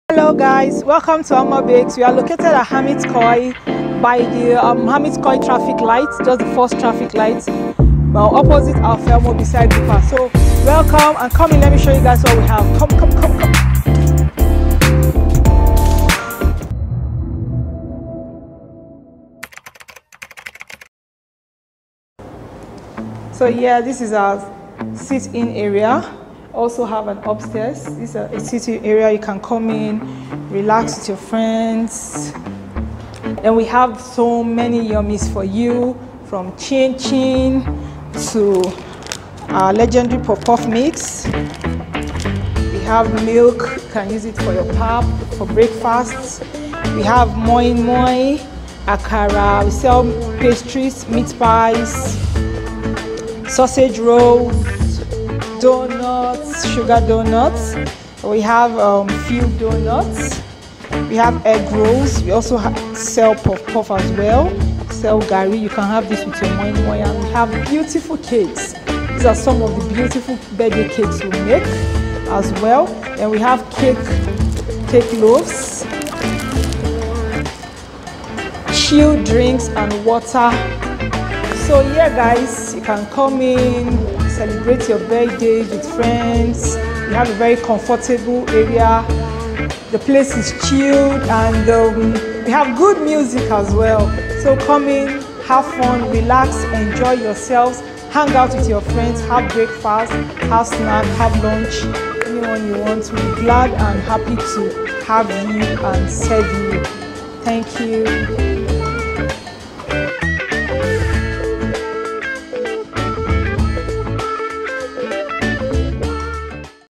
Ama Bakes - Bakery Shop Tour
A tour of the lovely Ama Bakes bakery shop in Nicosa, Cyprus